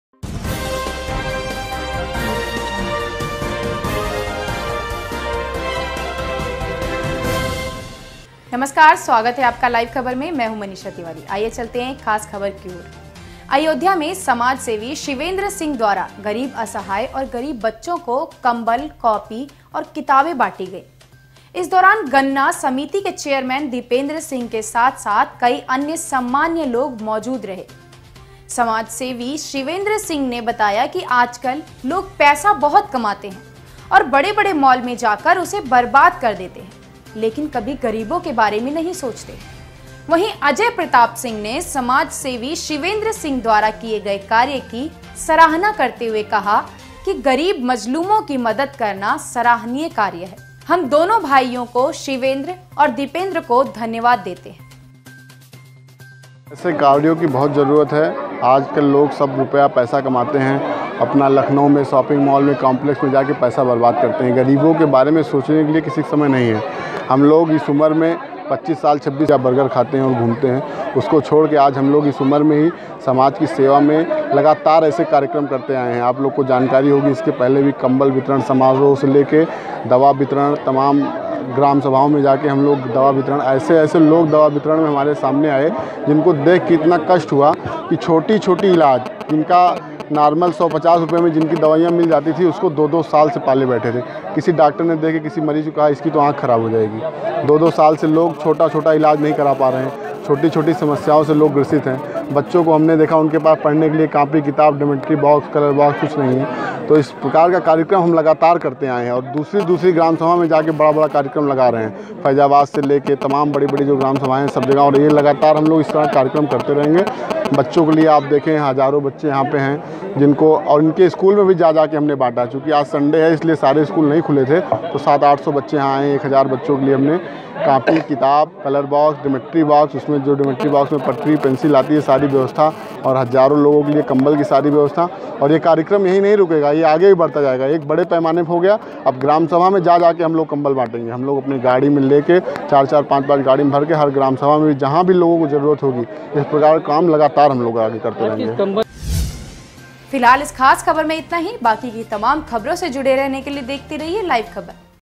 गरीबों और बच्चों को बांटे गए कंबल और कॉपी किताबें I ayodhya I
अयोध्या में समाज सेवी शिवेंद्र सिंह द्वारा गरीब, असहाय और गरीब बच्चों को कंबल, कॉपी किताबों बांटें गए... इस दौरान गन्ना समिति के चेयरमैन दीपेंद्र सिंह के साथ- साथ कई अन्य सम्मानिय लोग मौजूद रहे... समाज सेवी शिवेंद्र सिंह ने बताया कि आज- कल लोग पैसा बहुत कमाते है... और बड़े- बड़े मॉल में जाकर उसे बर्बाद कर देते है... लेकिन कभी गरीबों के बारे में नहीं सोचते है... वहीं, अजय प्रताप सिंह ने समाज सेवी शिवेंद्र सिंह द्वारा किए गए कार्य की सराहना करते हुए कहा कि गरीब मजलूमों की मदद करना सराहनीय कार्य है... हम दोनों भाइयों को शिवेंद्र और दीपेंद्र को धन्यवाद देते है...